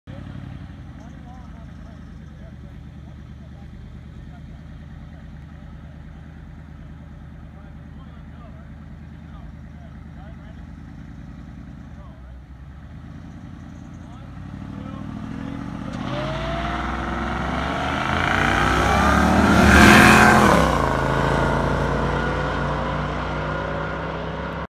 2017 Mustang GT vs 2003 Harley Davidson F150
Mustang is stock
F150 has a smaller pulley (making around the same boost as a stock lightning)

We did a bunch of racing beforehand, so the F150 was probably heat soaked by the time we got to this race